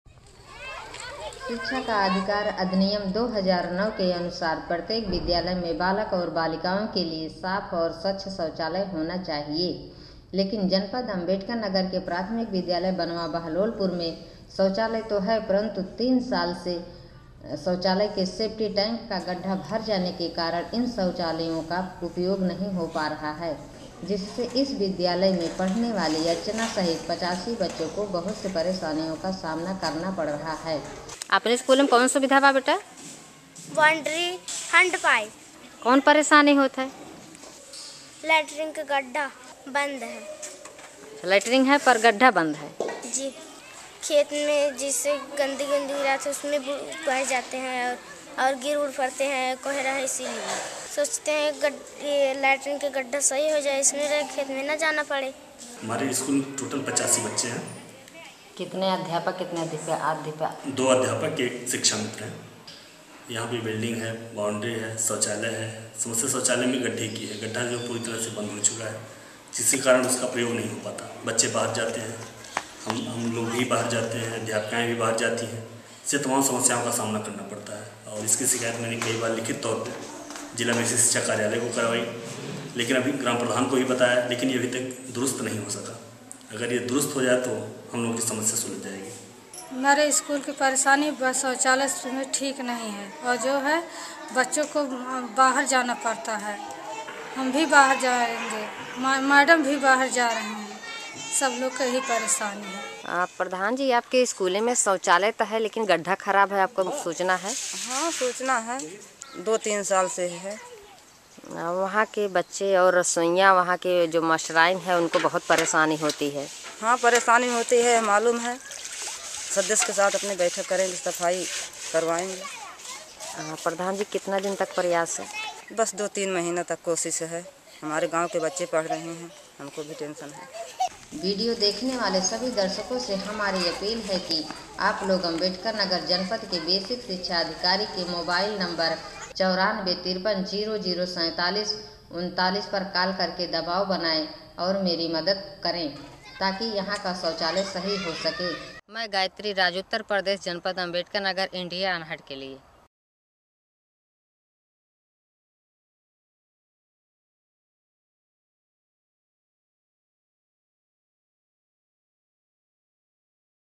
Toilet Problem in Banawa School, Uttar Pradesh— Video Volunteer Gayatri reports
This video was made by a Video Volunteers Community Correspondent Gayatri Devi.